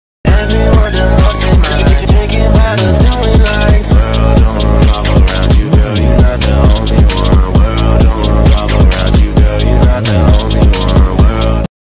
World don't revolve around you🌍//gacha club meme//King John and the magna carta//HISTORICAL//
Ok this do be kinda lazy and probably off timed a bit 💀 Because I rushed it a bit so I can get it out on the anniversary of the Magna carta 😭

which yes today is the 808th anniversary of the Magna carta being signed 📝✍️

HISTORICAL FIGURES MENTIONED:
John Lackland 🏴󠁧󠁢󠁥󠁮󠁧󠁿✍️

TIME TAKEN:
about two hours

APPS USED:
Gacha club
Ibis Paint X
Alright Motion

Hope you all have a great day/night :)